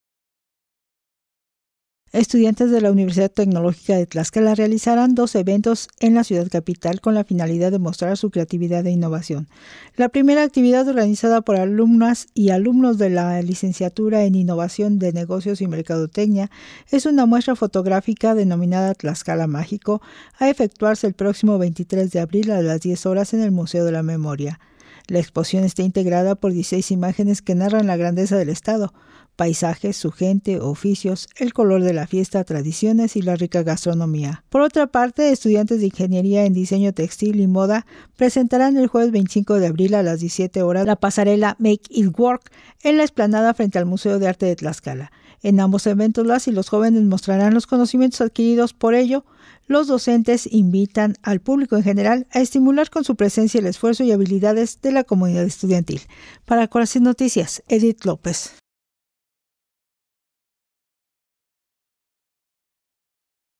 Invita UTT a dos eventos en la capital tlaxcalteca
La próxima semana habrá quna muestra fotográfica y una pasarela en los Museos de la Memoria y de Arte de Tlxcala, consulta los detalles en esta nota.